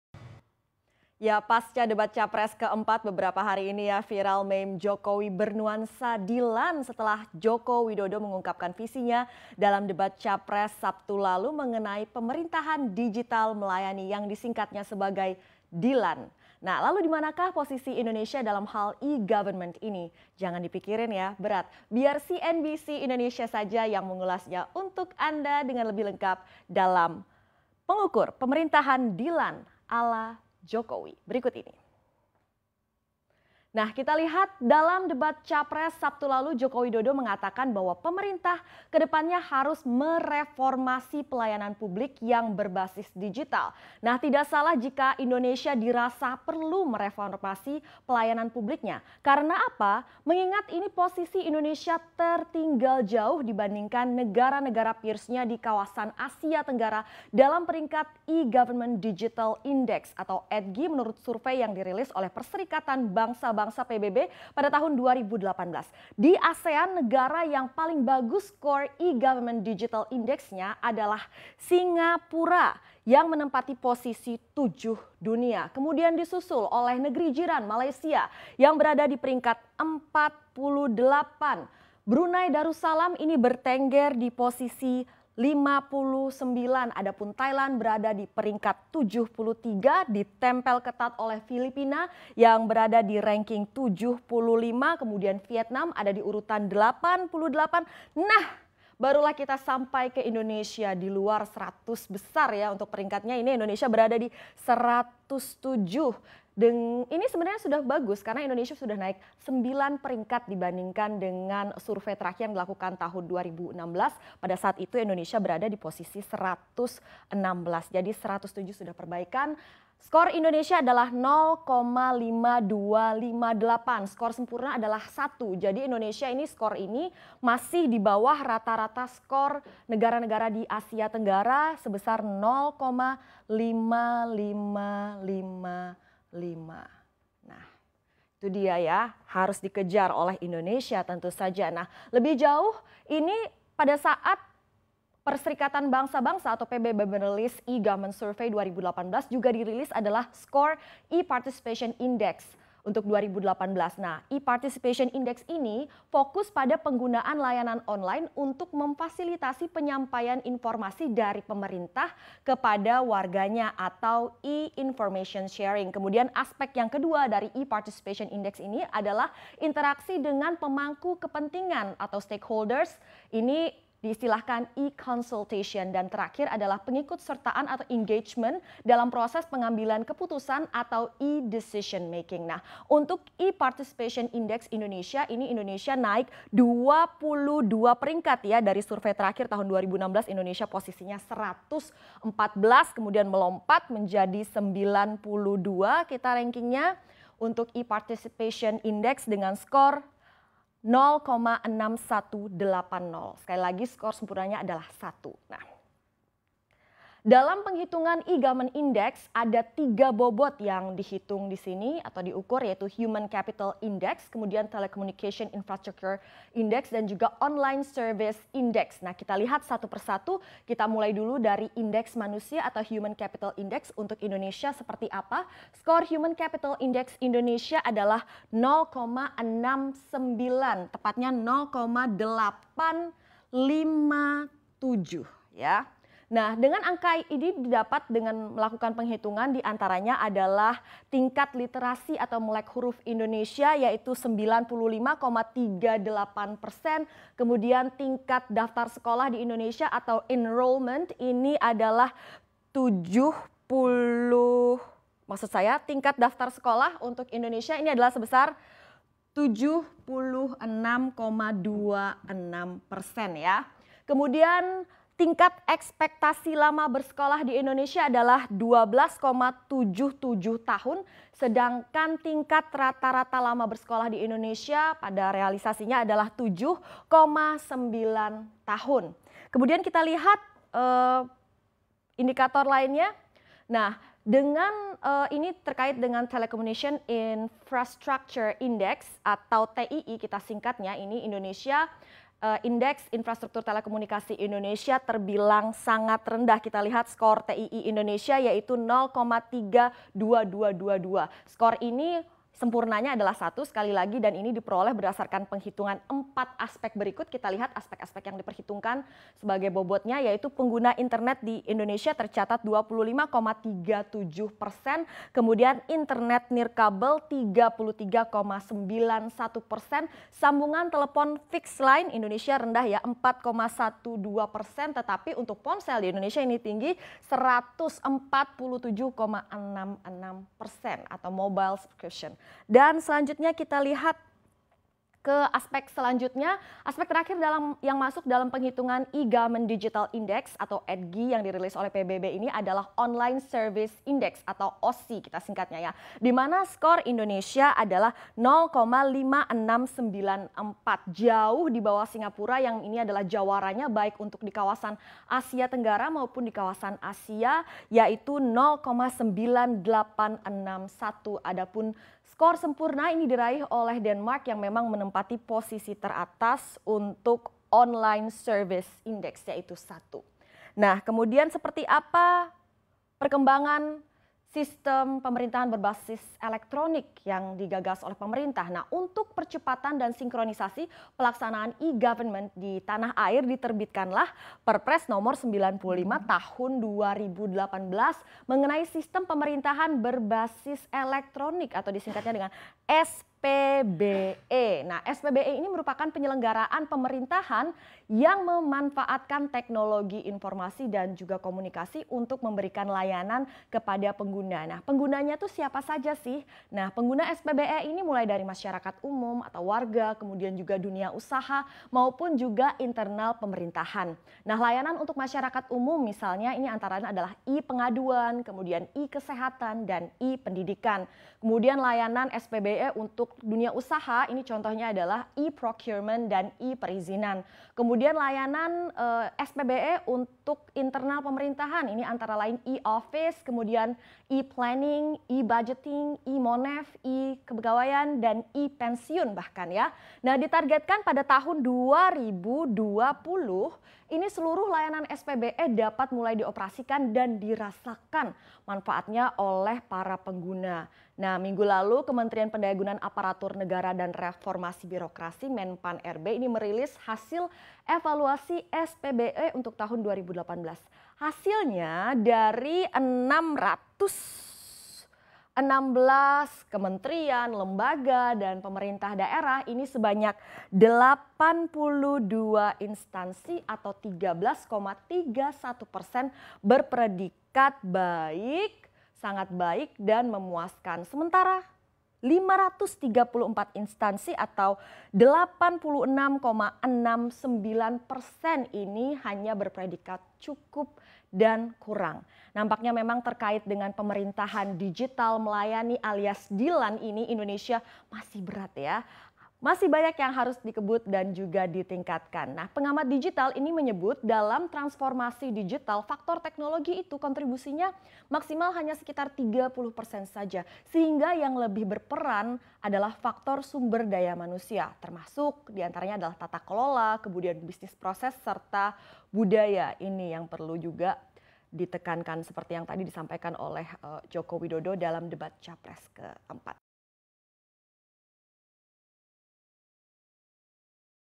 Ada "Dilan" di Pemerintahan
Beberapa hari ini viral meme Joko Widodo bernuansa Dilan, setelah dia mengungkapkan visinya dalam debat capres mengenai "pemerintahan digital melayani" yang disingkatnya sebagai Dilan.
Lalu, di manakah posisi Indonesia dalam hal e-government ini? Simak ulasan lengkap bersama dengan Aline Wiratmaja di Profit, CNBC Indonesia, Senin (1/4/2019).

Like & Follow Facebook:
CNBC Indonesia

cnbcindonesia